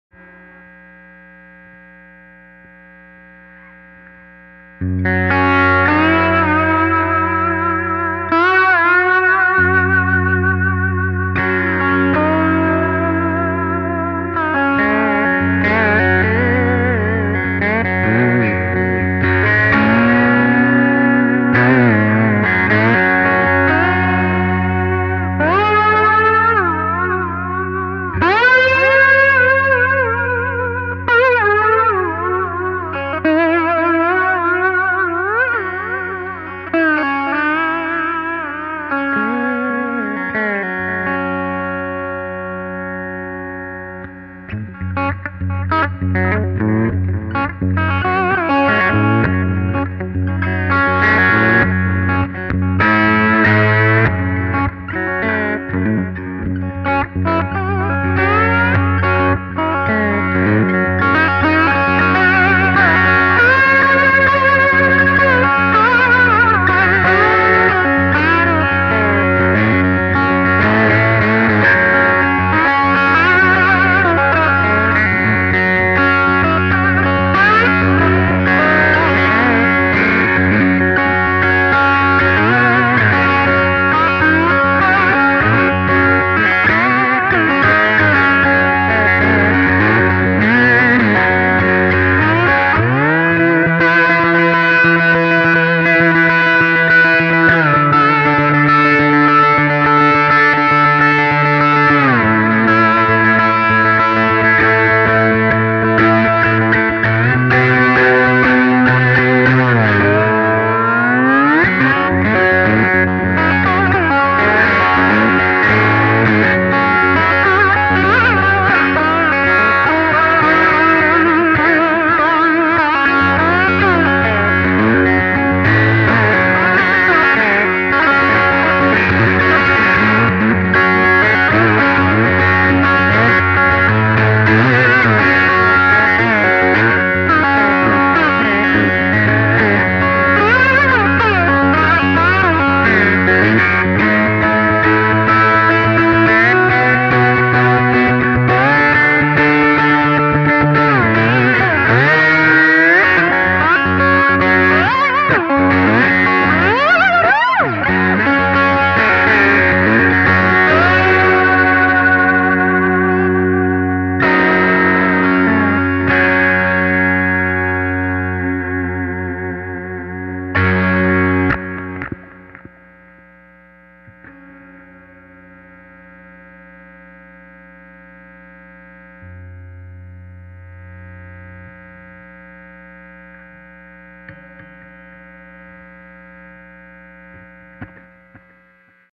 Dulcimer/Lap Steel Hybrid Prototype w/ P-90 pickup
I found some maple planks in the trash room at my apartment building and decided to put one to use today. I built this crazy contraption this afternoon using whatever I had lying around my room. It has a set of Gotoh ukulele tuners, an old home-made saddle used for the nut, a Dunlop bullet bar for a saddle on a Strat bridge plate, and a Mighty Mite P-90 wired straight to a jack inside an Altoids can. It's strung F# F# C#C# (top two in unison pair, basically like a dulcimer but different tuning) on a 24" scale length.